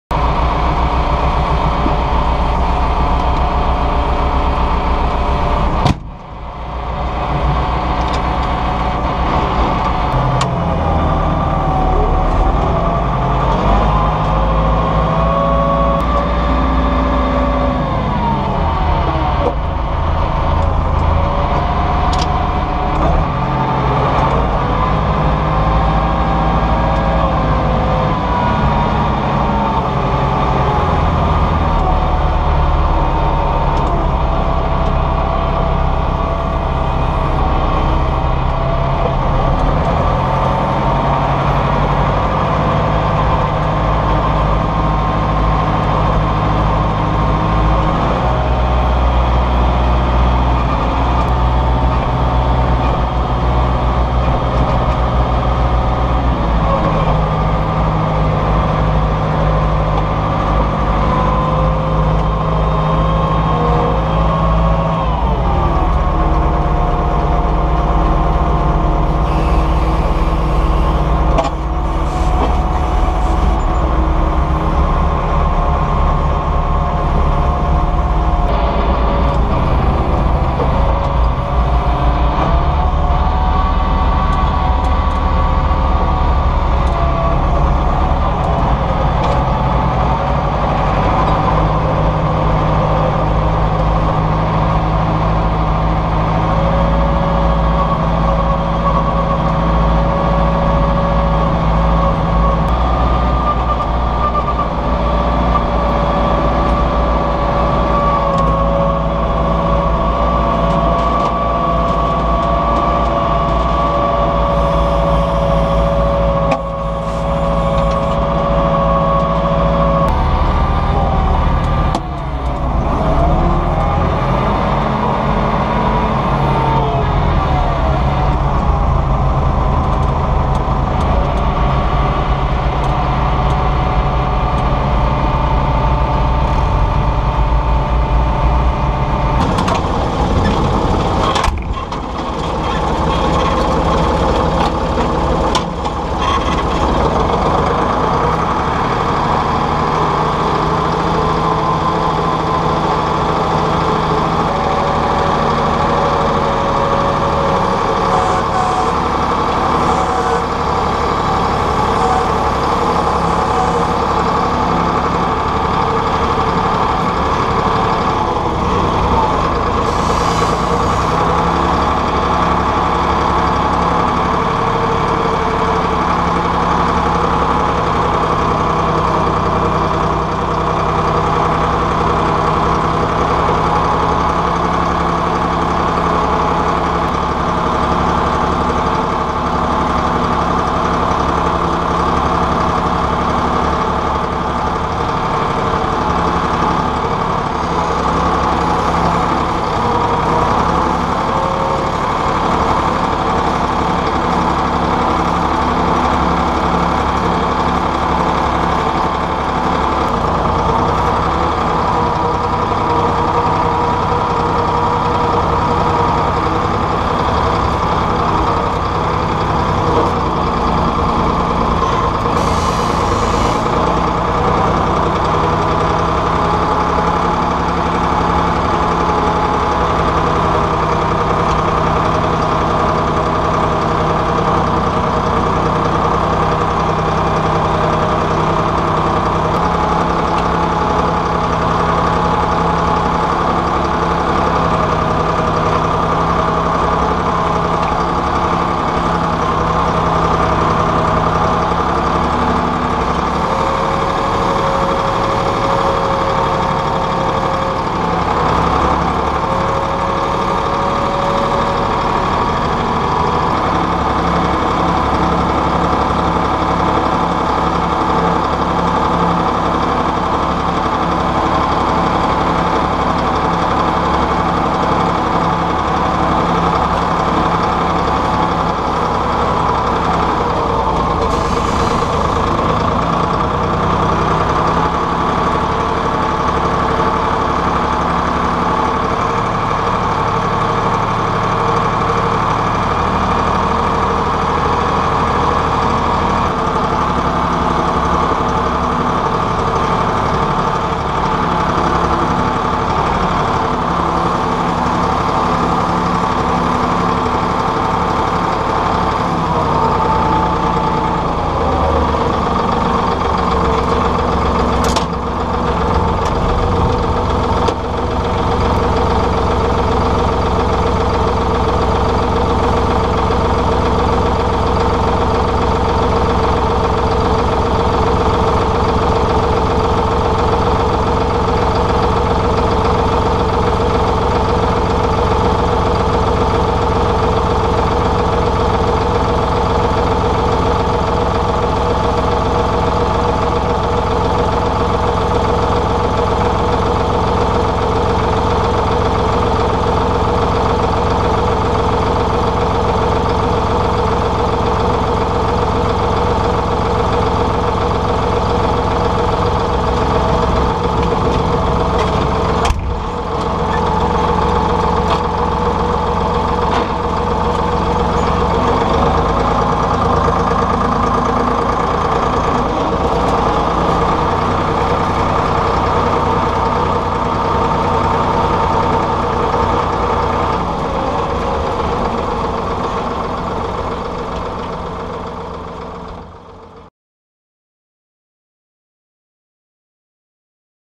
Scottish County Tractor Club, in cab ploughing 2, Oct 2014
The Scottish County Tractor Club working week-end 11-12 October 2014. Oldmeldrum, Aberdeenshire.